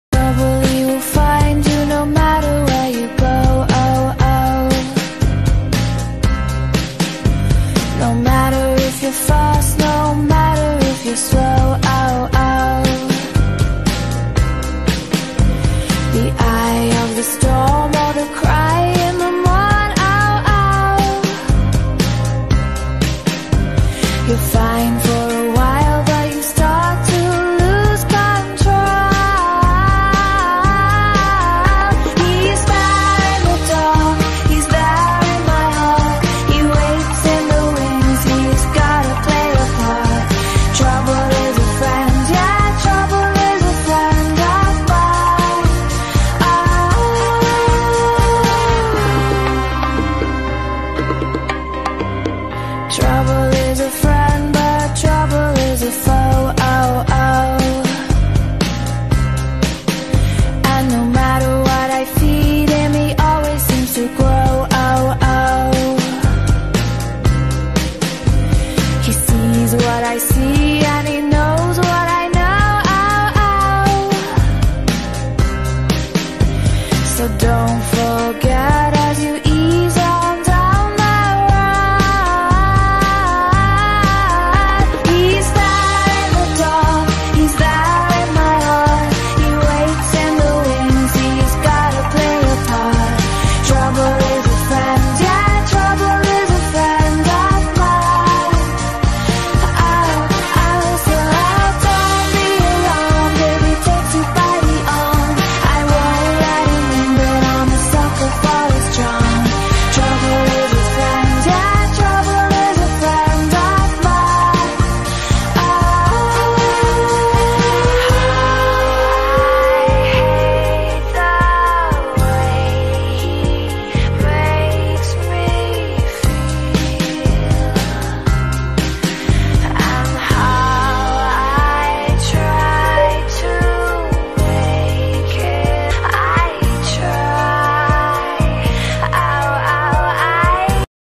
Data Recovery Repair 影樓RAID5掉線，客戶急需照片，可把樓主急壞了！
古诚数据恢复维修中心